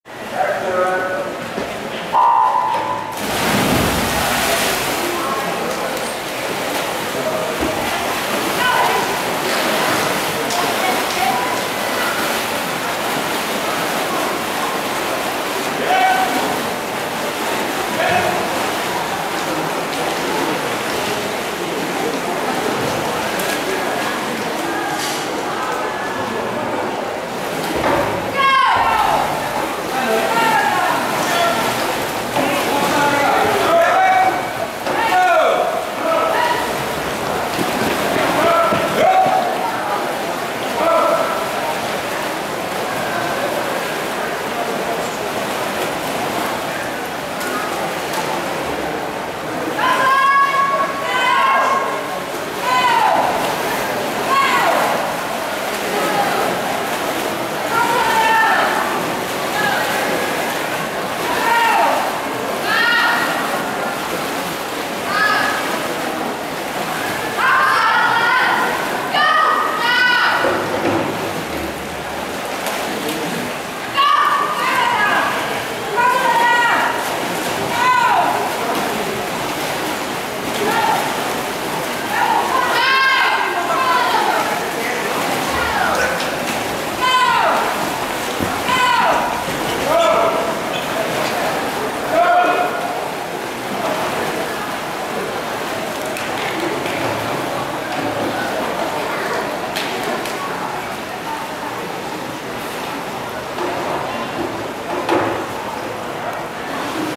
SMS Jólastevna 2012 - 100 bringa kvinnur - riðil 1 av 2
Geil 1 - Krista Mørkøre (1998) - Ægir
Geil 2 - Lena Rannváardóttir (1998) - HS
Geil 3 - Hanna Lervig (2000) - Reysti
Geil 4 - Ronja Eysturdal (1999) - Ægir
Geil 5 - Helga Nielsen (1998) - FLOT
Geil 6 - Vár E Eidesgaard (2001) - HS